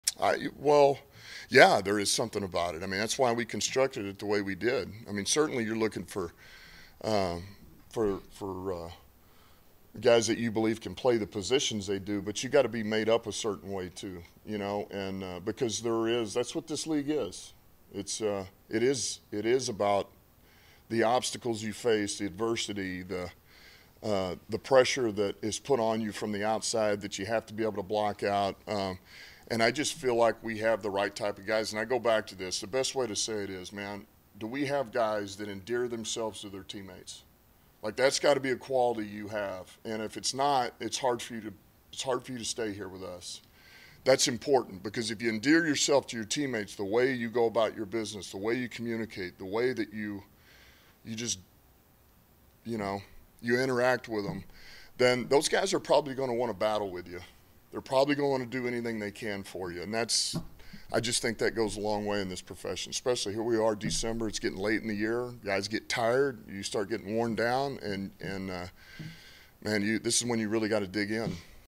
The Lions have not lost back-to-back games all season — Dan Campbell believes that’s no accident
“Yeah, there is something about it. I mean, that’s why we constructed it the way we did,” he said.